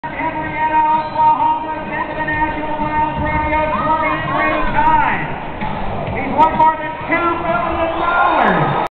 Accion rapida=Jalisco.
Esta bato ato a este  toros como si fuera un beserro y piensa que es  grande e inportante como la gente de jalisco digo porque los de Atoyac somos perfectos pero cuando ablamos de  raza de los Laureles aqui puro serebre mi amigo